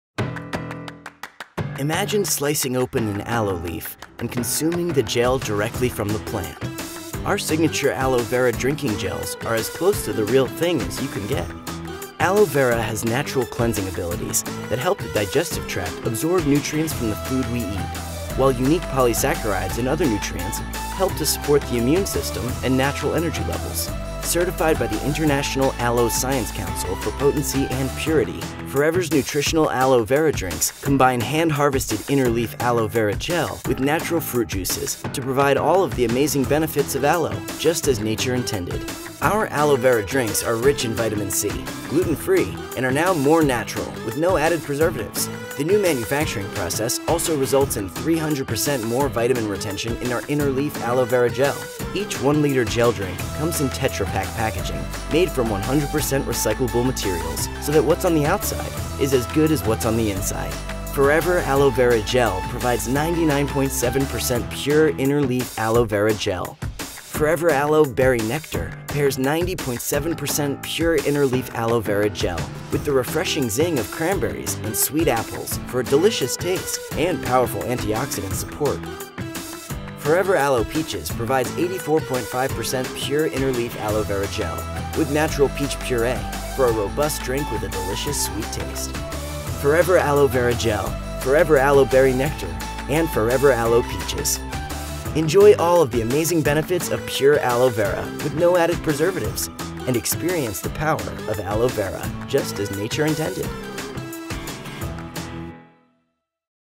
Forever Living Aloe Vera Gel Drinks
Imagine slicing open an aloe leaf and consuming the gel directly from the plant. Forever Living's signature aloe vera drinks are as close to the real thing as you can get. Supports healthy digestion. Promotes a healthy immune system. Helps maintain natural energy levels. Assists natural beauty.